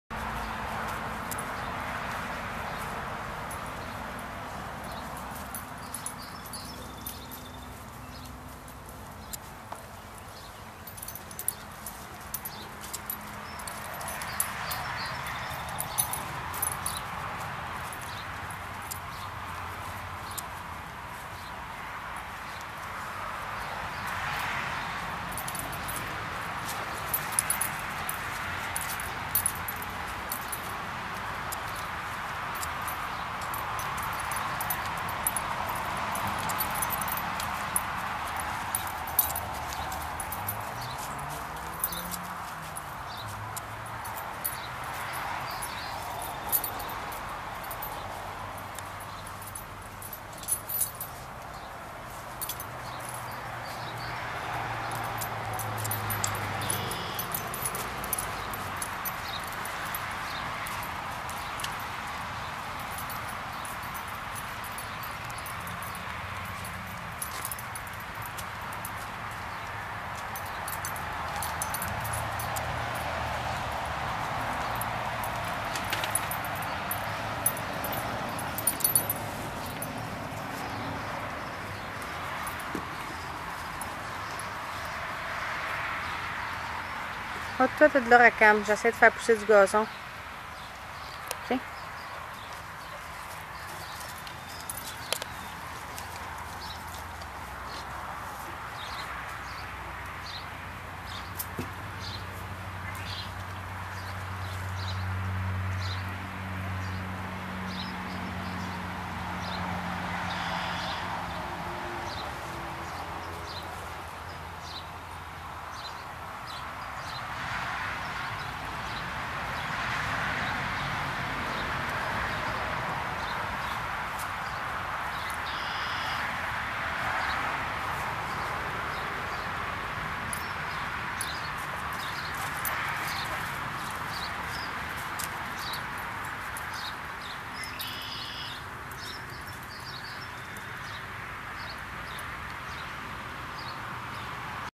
Démarche ski de fond Rakam mai 2009
Sur ce vidéo, on voit bien la démarche de Rakam en ski de fond comme l'appele les neurologues... Dû à la dégénérécence de sa moelle épinière...